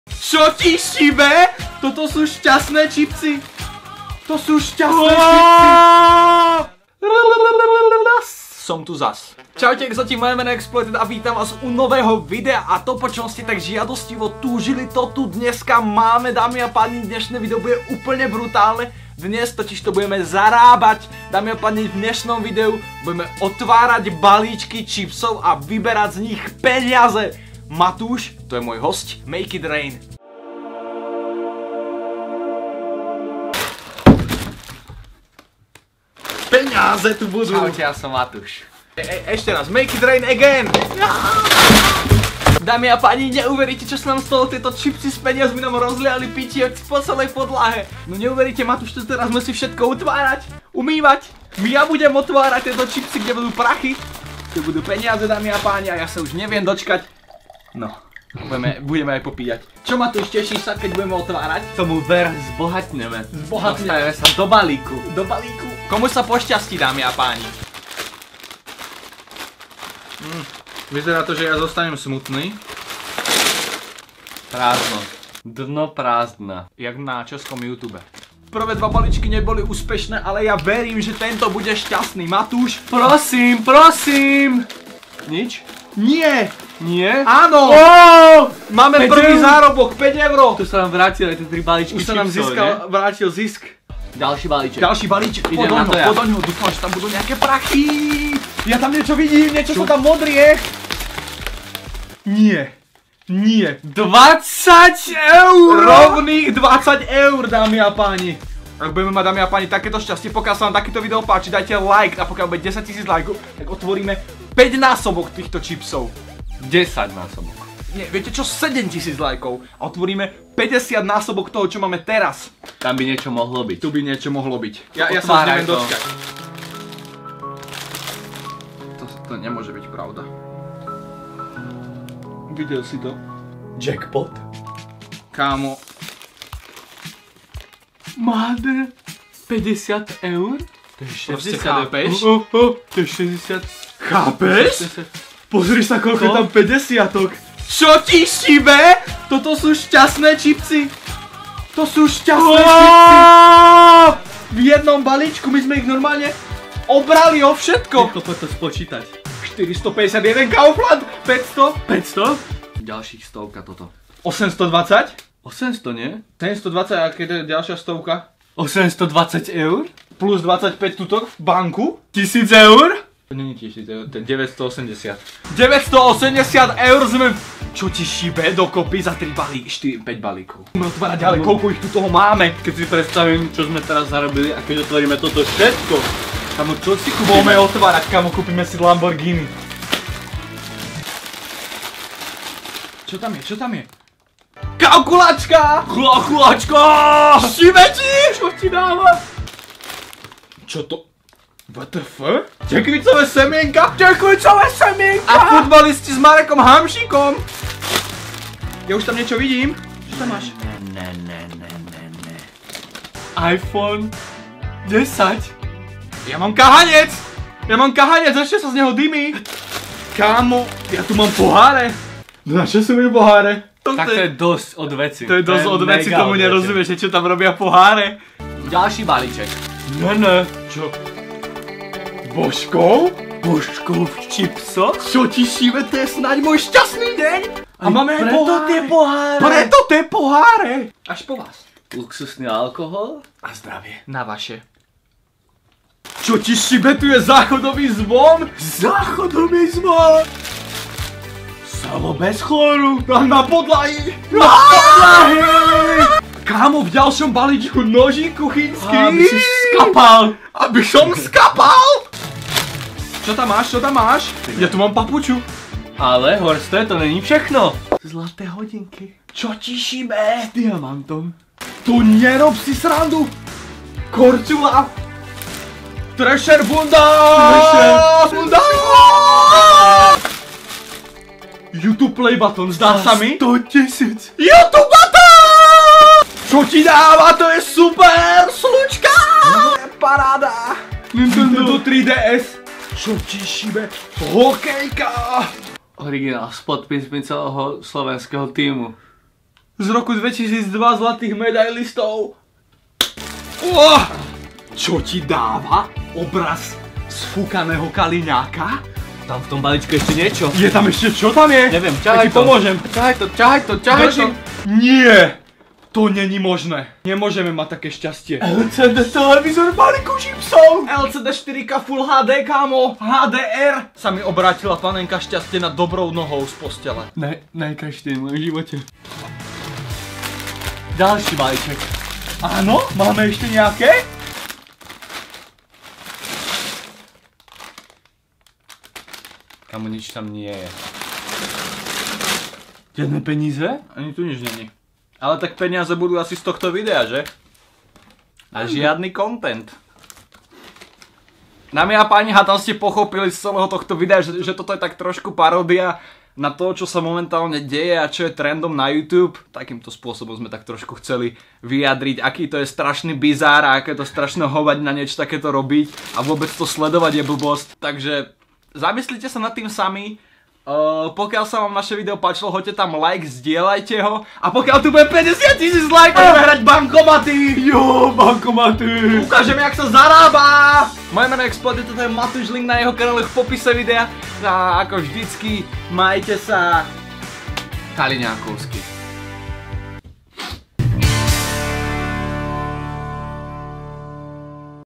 Našli sme 980 EUR v Balíkoch CHIPSOV!
Dnes vám ukážeme sme si ľahko zarobili! Budeme hľadať peniaze v balíčkoch od chipsov a nájdeme výhru!!!